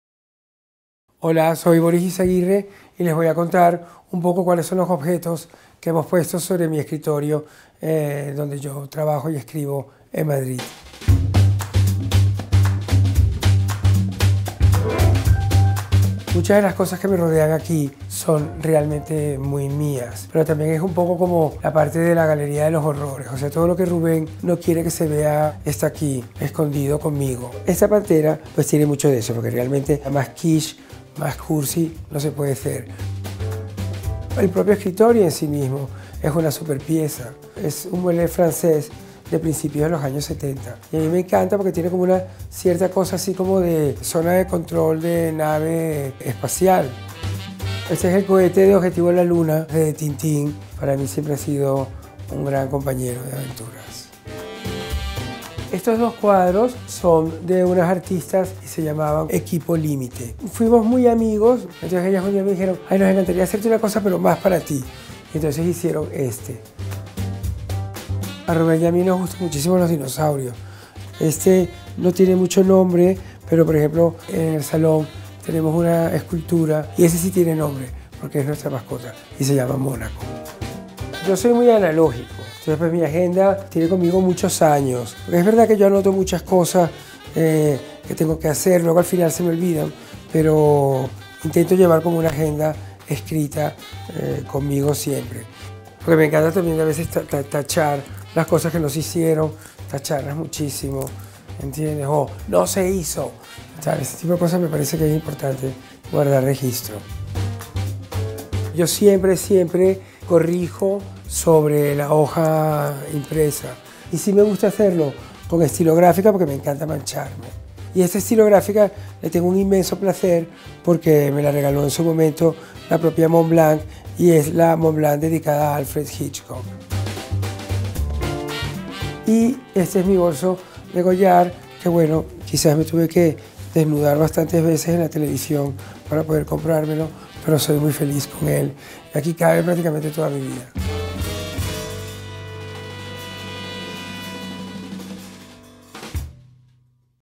El escritorio de Boris Izaguirre
El escritor nos enseña su "galería kitsch"